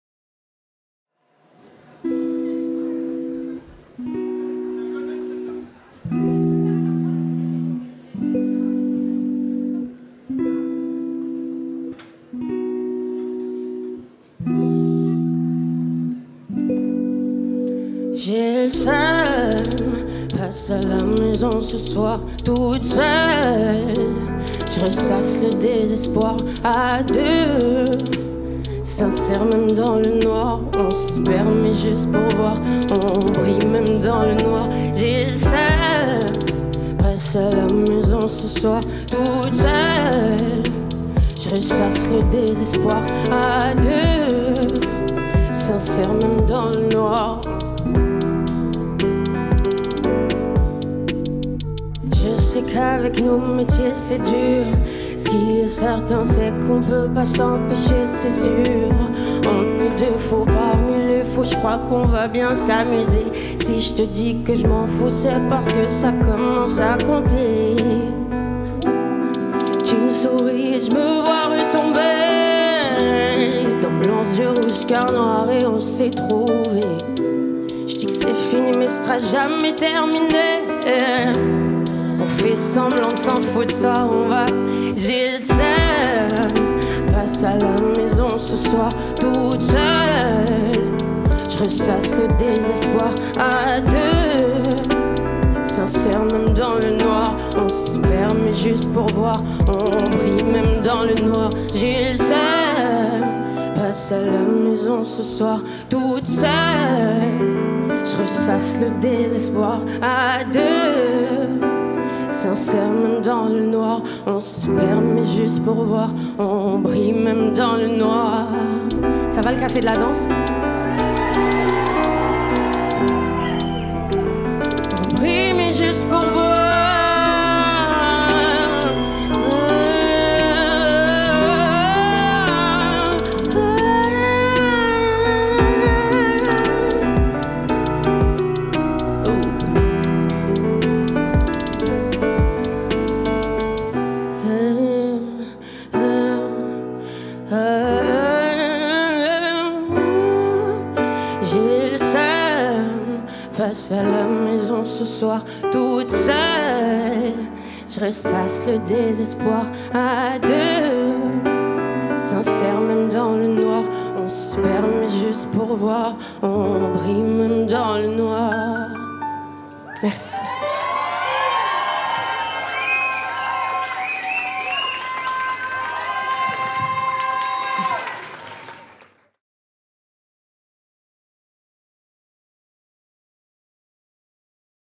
au Café de la Danse / fair 2022 session #1
Le fair présente :
fair 2022 session #1 / Reÿn au Café de la Danse
Concert enregistré le 28 février 2022 à Paris par Couac Productions

Réalisation et montage : Clément Vallos
Image : Hugo Bachelet / Charles Devoyer / Victor Bruzzi / Adrien Mendoza / Vincent Laisney / Matthieu Yakovleff
Son : Perceval Carré
Production : Céline Beillon

Le fair : Julien Soulié / Julie Cerizay / Fabienne Azzaro / Patricia Teglia
Café de la Danse : Loïc Barrouk / Matthieu Blestel / Yohann Amsellem / Florie Hébrard